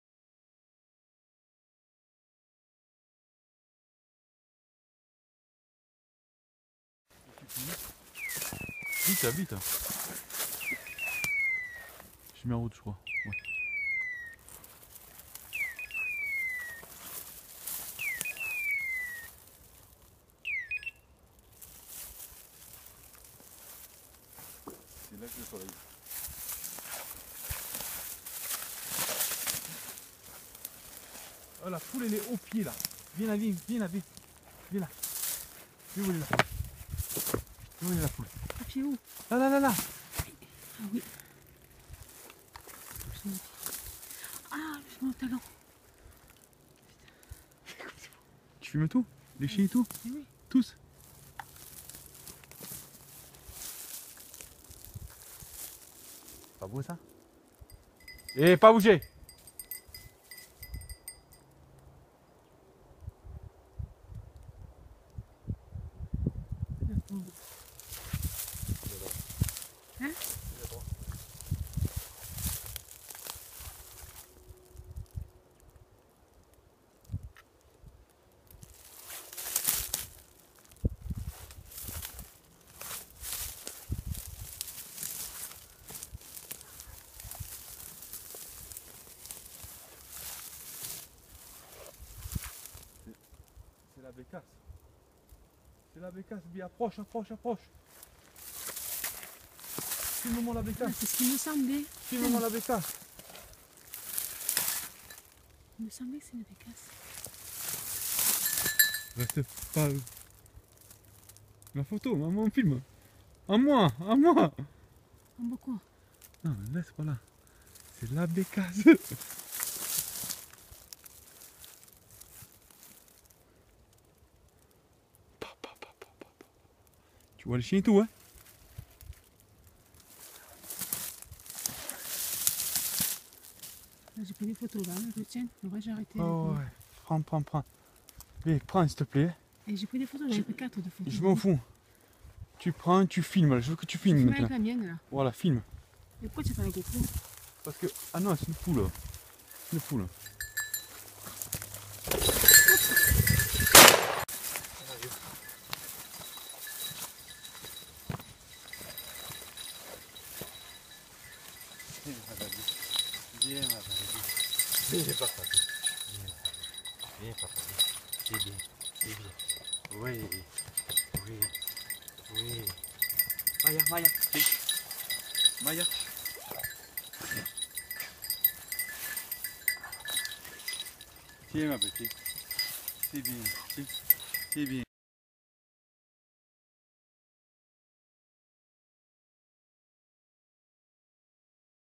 chasse faisan avec setter LE 01 01 2016 video gag !!!!
berti david : une chasse exceptionnel avec ma femme, elle film avec son téléphone (vidéo gag ! ) ma gopro plus de batterie